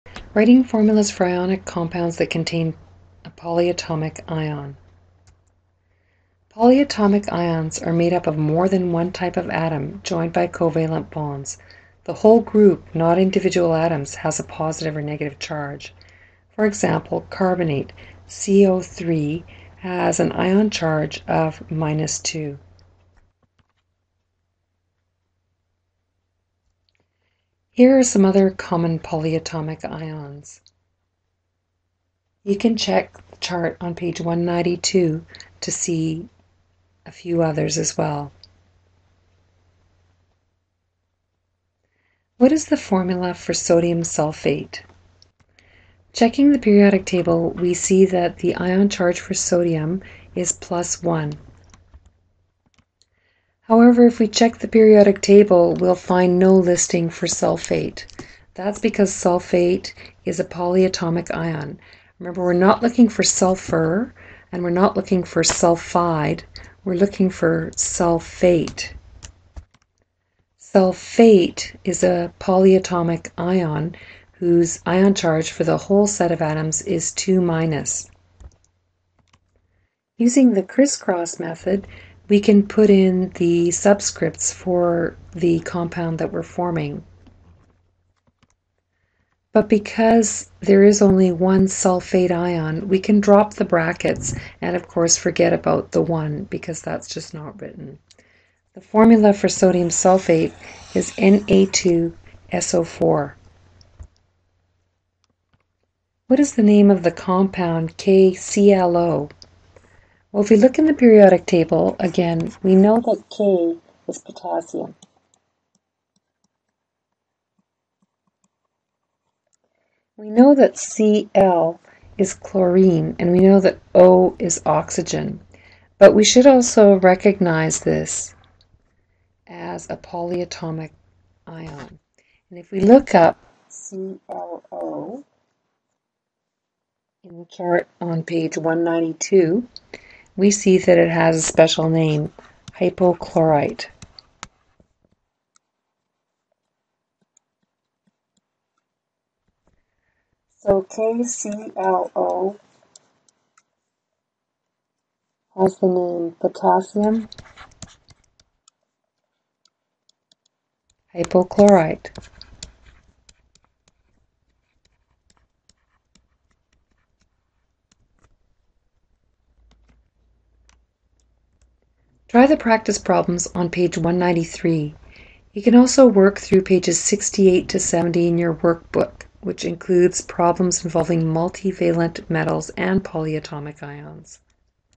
7. Writing Formulas for Compounds with Polyatomic ions
Check the table of polyatomic ions.  You should be able to recognize a polyatomic ion when you see one.